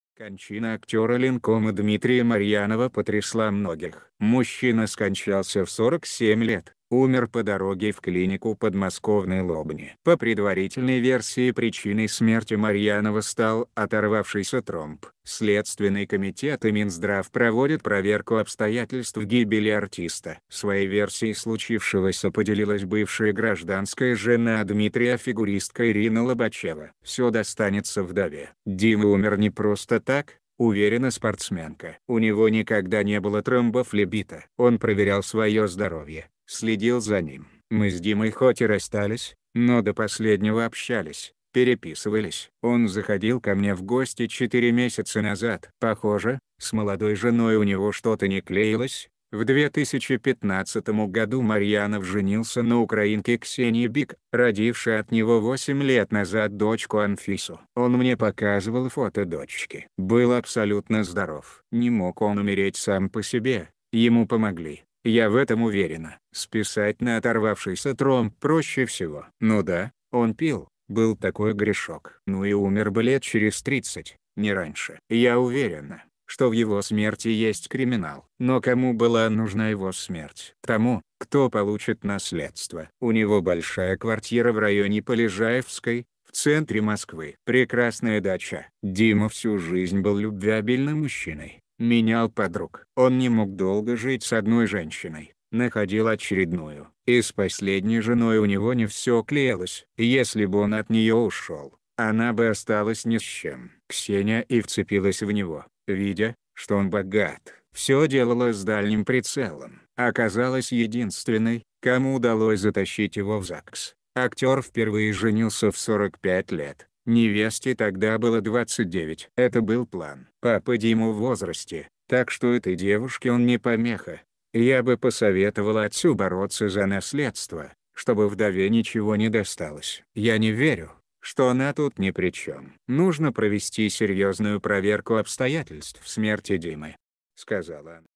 Подруга Марьянова обвинила жену актера
Подруга Дмитрия Марьянова фигуристка Ирина Лобачева обвинила его жену Ксению Бик в борьбе за наследство.